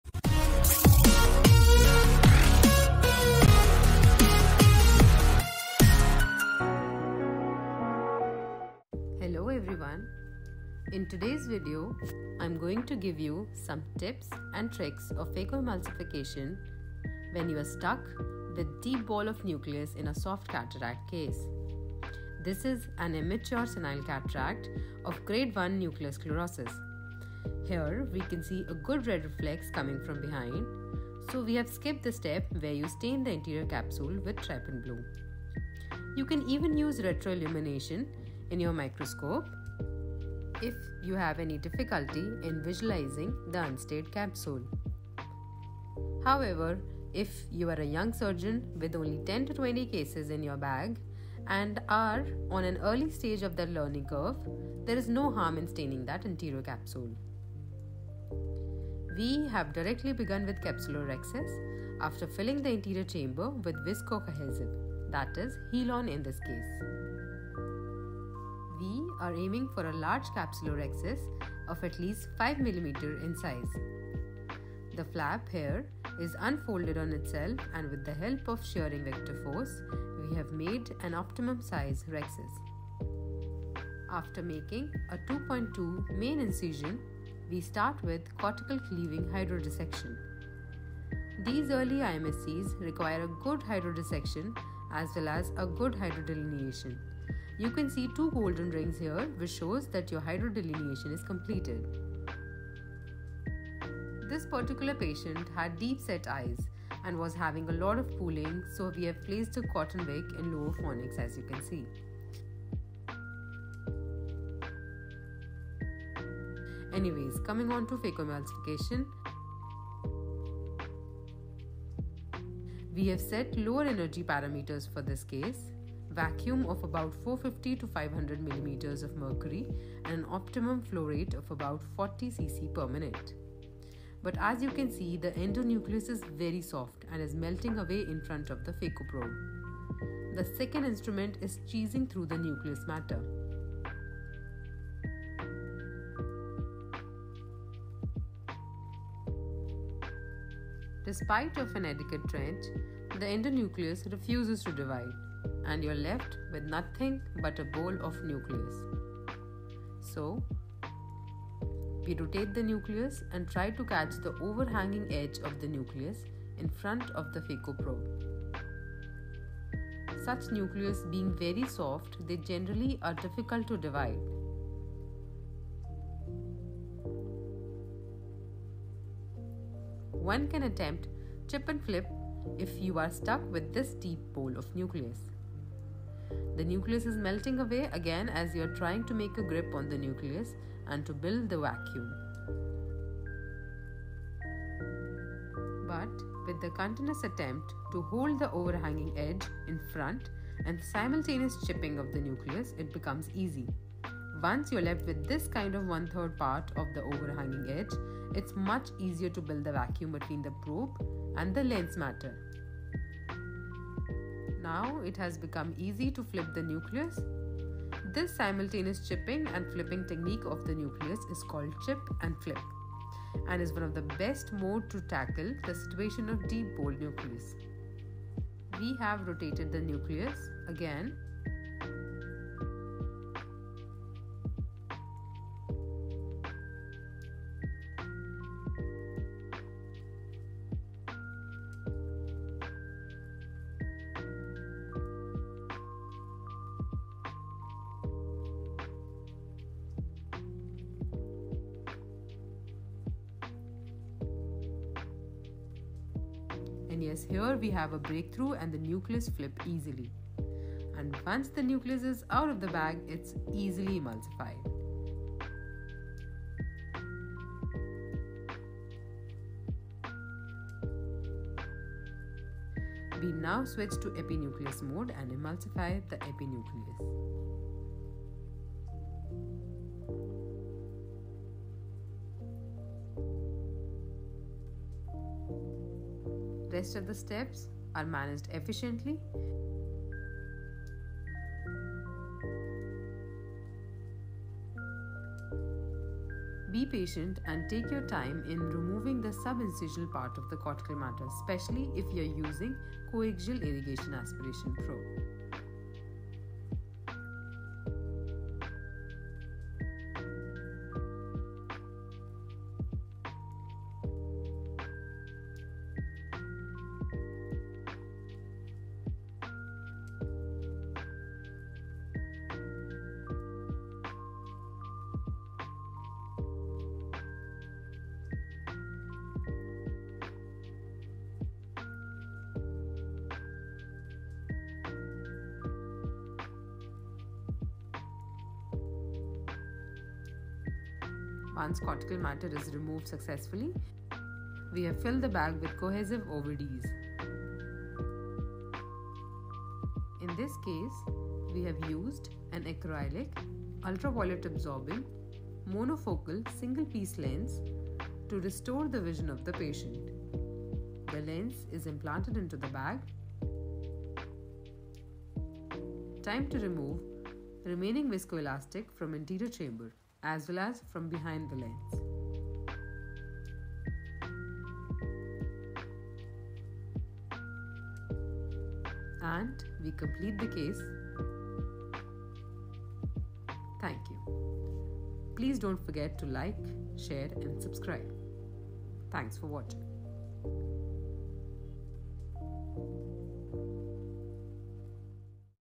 Mastering Chip & Flip technique in Phacoemulsification of soft cataracts
With increasing access to healthcare, the number of patients with early nuclear sclerosis and soft cataracts have also shown rise in our daily practice.

For such early IMSCs and softer cataracts Flip & Chip is one of the techniques which has proven its benefit time and again. In Grade I cataracts, lower energy parameters are needed. Effective phaco time has to be lower in order to minimise the endothelial cell loss.

In this video, I have described an effective way to learn and master the technique of Flip and Chip @lenscor @UdayDevgan @DrDeepakMegur